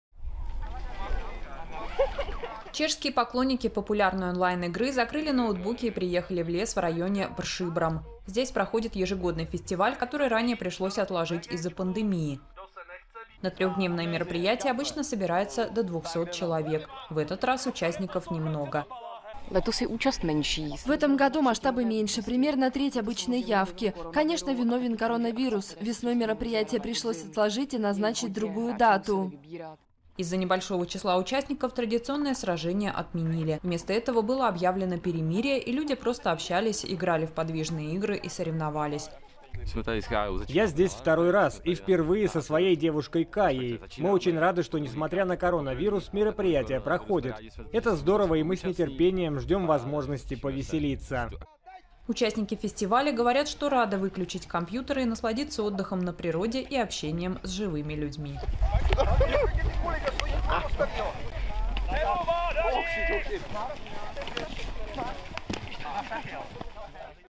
Поклонники онлайн-игры провели фестиваль в чешском лесу
Чешские поклонники популярной онлайн-игры провели фестиваль в лесу. Вместо обычных сражений они общались и участвовали в соревнованиях.